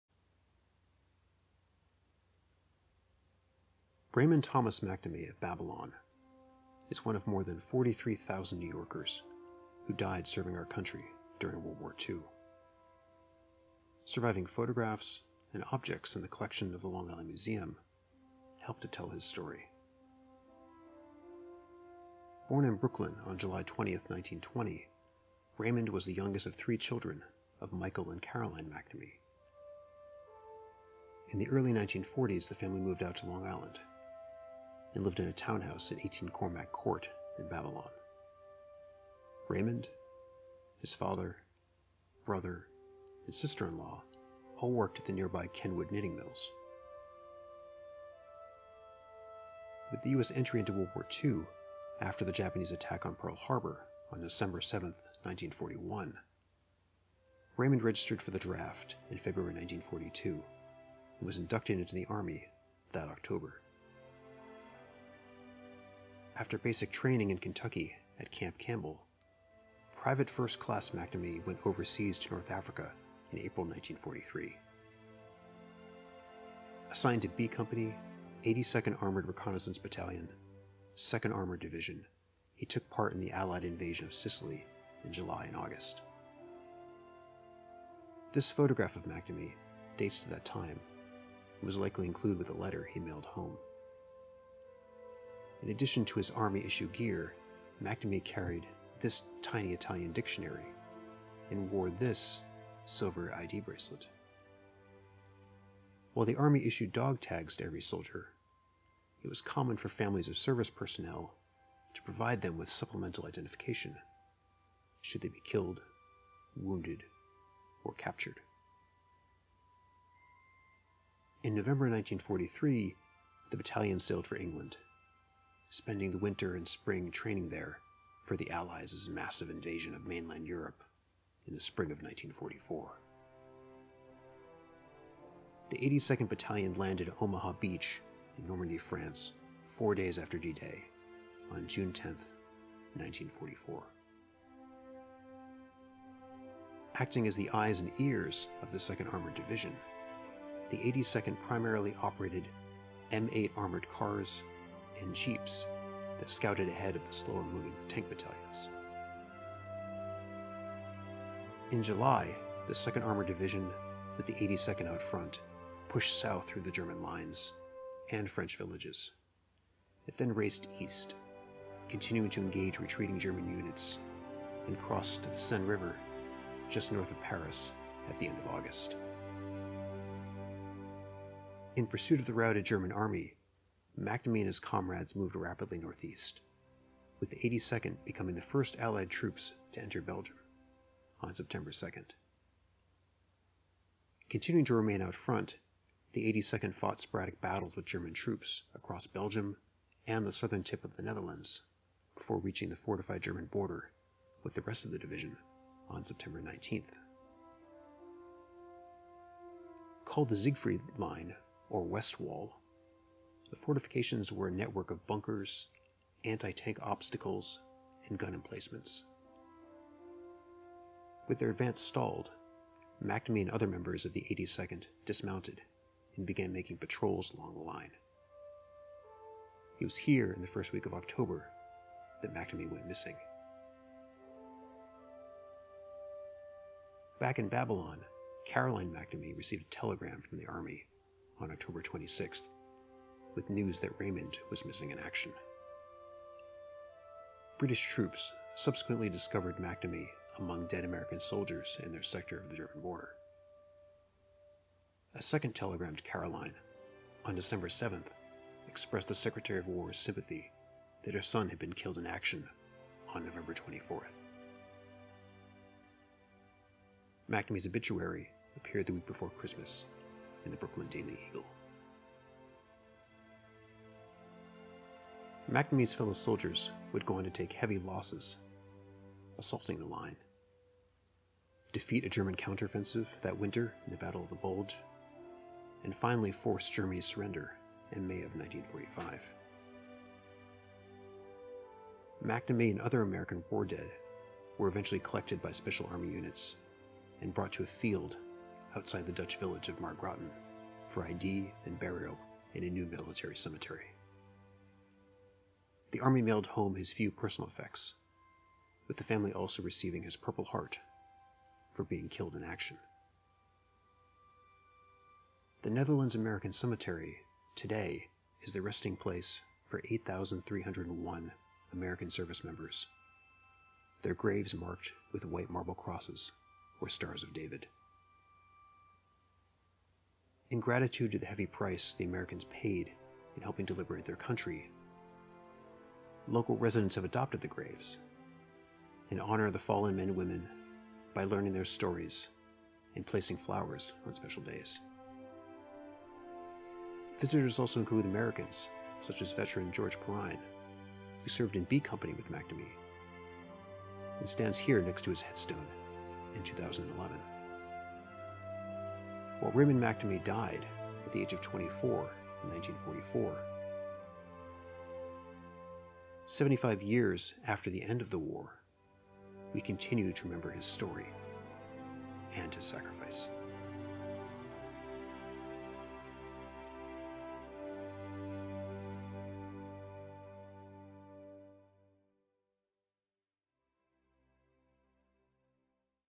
One Long Island Soldier: Remembering Raymond McNamee
Raymond T. McNamee of Babylon, Long Island, was drafted into the US Army during WWII. As a member of B Company, 82nd Armored Reconnaissance Battalion, 2nd Armored Division, he participated in the Allied invasion of Sicily in 1943, landed at Omaha Beach four days after D-Day, on June 10, 1944, and helped to liberate France, Belgium, and the Netherlands from German forces. He was killed in action in Germany on November 24, 1944, and is buried at the Netherlands American Cemetery just outside the village of Margraten. He is one of more than 43,000 New Yorkers who died while serving their country during World War II. Photographs and personal effects that tell the story of McNamee’s service and sacrifice are part of the collection of the Long Island Museum.